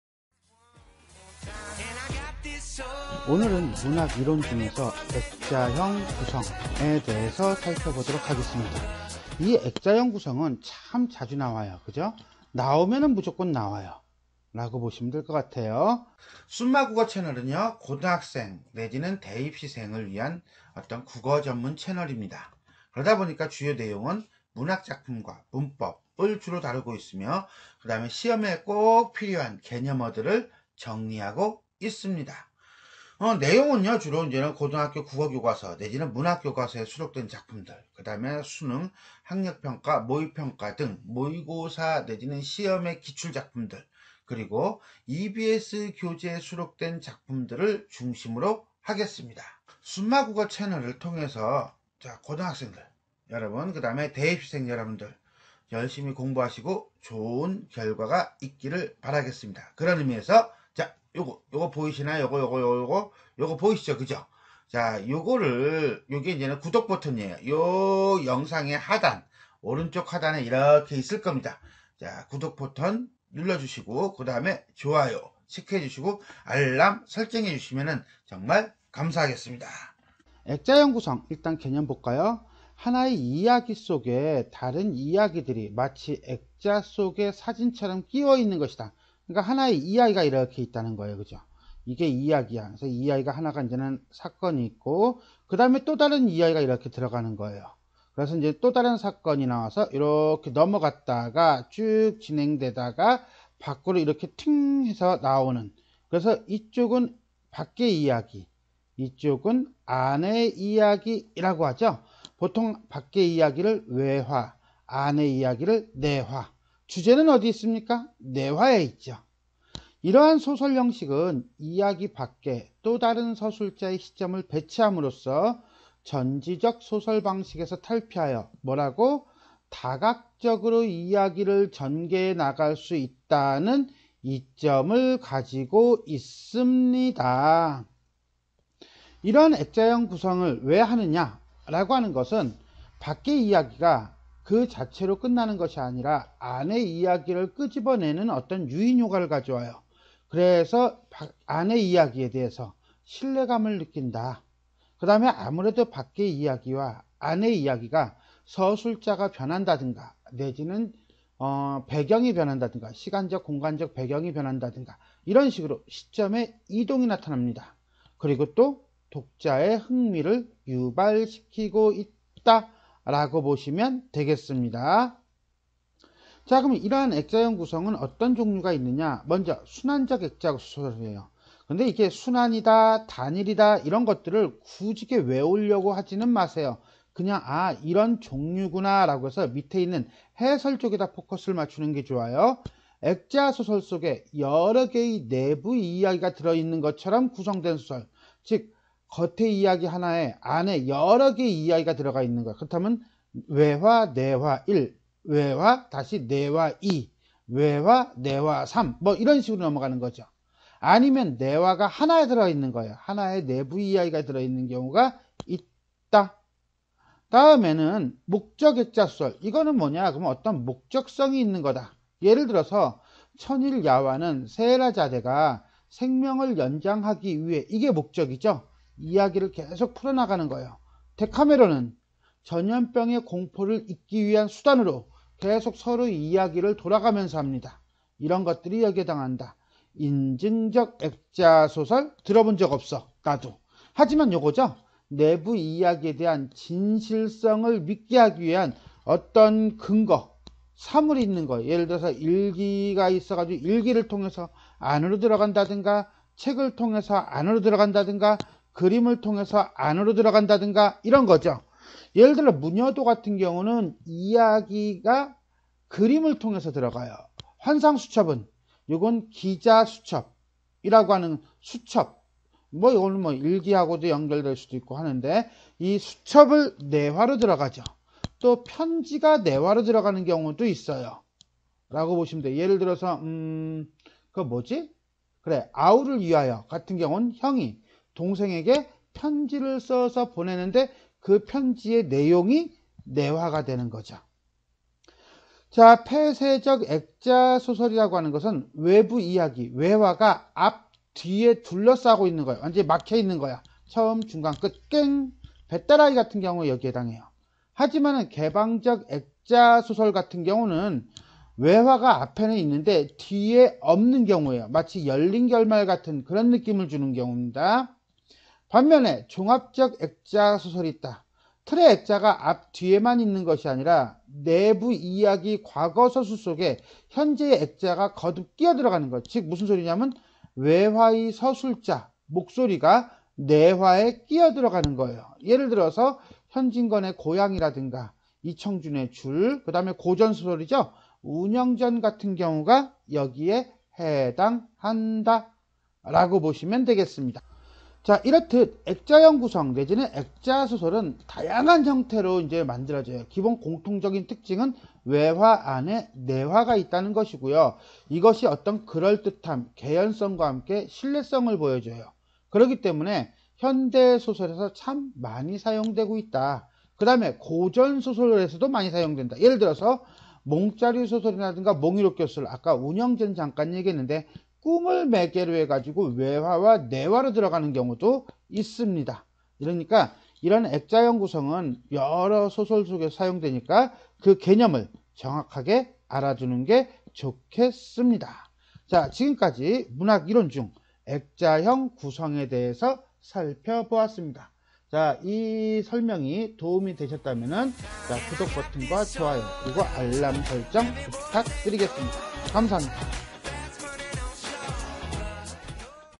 [문학 이론] 액자형 구성
'액자형 구성'은 고전, 현대 소설을 막론하고 다양한 양식으로 나타나는 구성입니다.  [더보기를 누르세요]

그리고 안나오면 모를까 나오면 꼭 출제되는 요소입니다. 그렇기 때문에 '액자형 구성'의 개념과 효과 그리고 유형에 대해 꼭 살펴보시기 바랍니다.

* 타임 라인 *
00:00   시작
01:20   '액자형 구성'이란
02:15   '액자형 구성'의 기능
02:55   '액자형 구성'의 유형